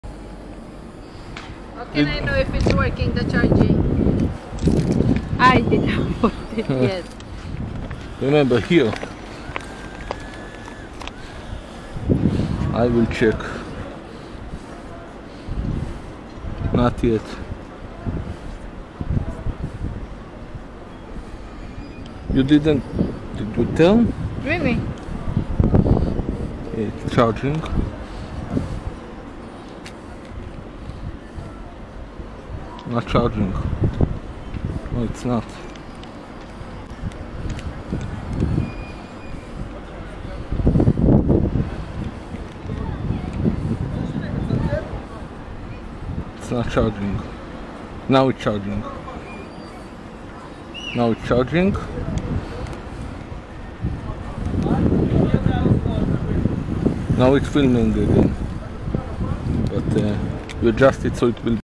2024 10 18 - טעינת מצלמת גוף, מהחשמל של האופניים - Charging body camera from electric bicycle
חיברתי את מצלמת הגוף של בת הזוג שלי, לשקע המצית 12 וולט, שהתקנתי לה באופניים, כי אחרי 3 שעות של טיול בדרום ת"א ויפו, התרוקנה לה הסוללה.